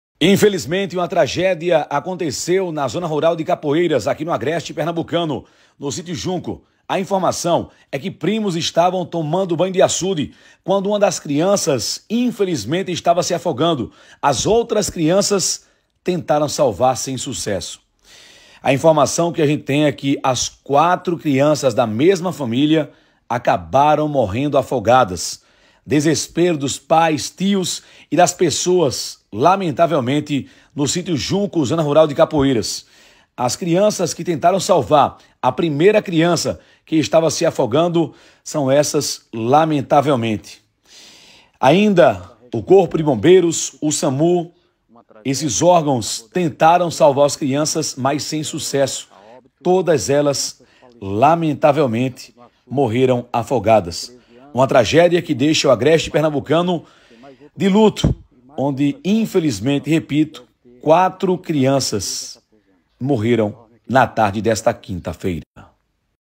Tragédia: 04 crianças morreram afogadas numa barragem na zona rural do município de Capoeiras
tragédia Comoção e tristeza tomaram conta dos moradores de Capoeiras, no Agreste de Pernambuco, nesta quinta-feira 02/11/2023. Na tarde de hoje, três crianças e uma adolescentes de 14 morreram afogadas numa barragem localizada no Sítio Junco, zona rural do município.